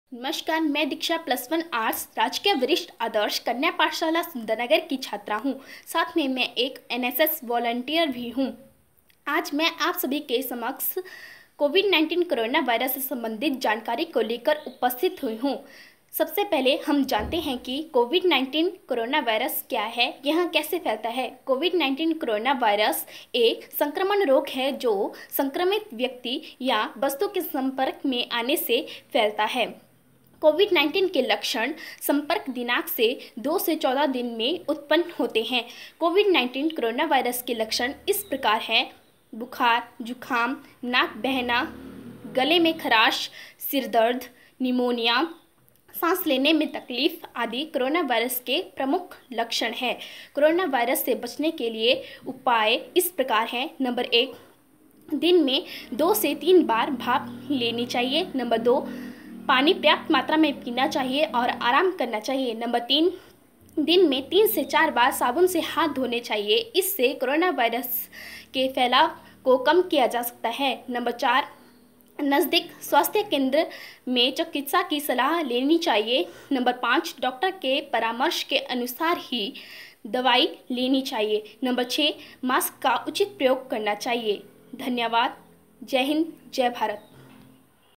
NSS Volunteer Miss Diksha GMSSS Girls SunderNagar Mandi H.P expressing her views on COVID19 Pandemic
NSS Volunteer Miss Diksha GMSSS Girls SunderNagar Mandi H.P expressing her views on COVID-19 Pandemic...